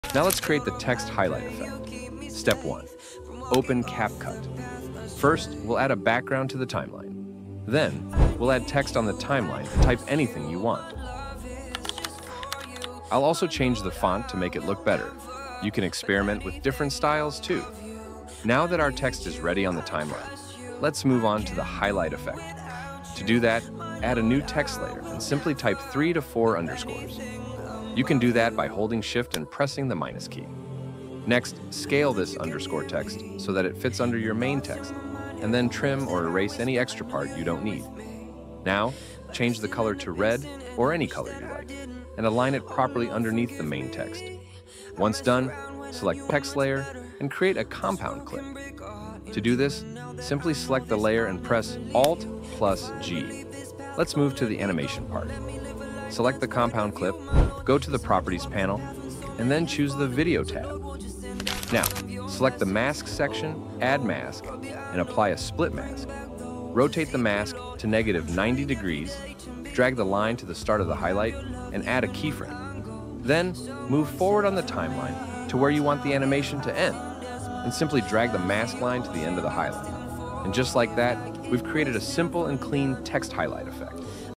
Easy Text Highlight effect in Capcut
Learn how to make text highlights in CapCut like a pro in just 2 minutes! This tutorial will show you how to create a text highlight effect for your videos using CapCut. Master the art of highlighting text and documents in your videos with this simple and easy tutorial. Perfect for beginners looking to add a professional touch to their videos!

Perfect for video editors, content creators, and students who want to make their content look more professional and clean.

🔧 What you'll learn in this video:
✔ How to add a glowing text highlight in CapCut
✔ How to highlight lines in a document (paper/newspaper style)
✔ Easy animation using masking & keyframes
✔ Tips to make your edits clean and modern

text highlight capcut,
how to add background text in capcut